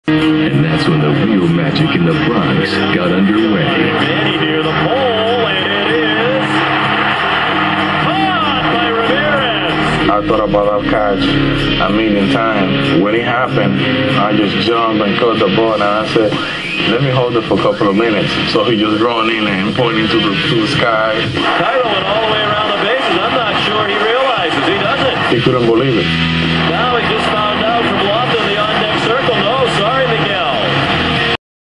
Boston Red Sox Manny Ramirez Robs Home Run At Yankee Stadium!
Boston Red Sox Manny Ramirez robbed a home run away from utility guy Miguel Cairo at Yankee Stadium in 2004! The best part of the clip is Miguel Cairo circling around all the bases before finally realizing that Manny caught the ball!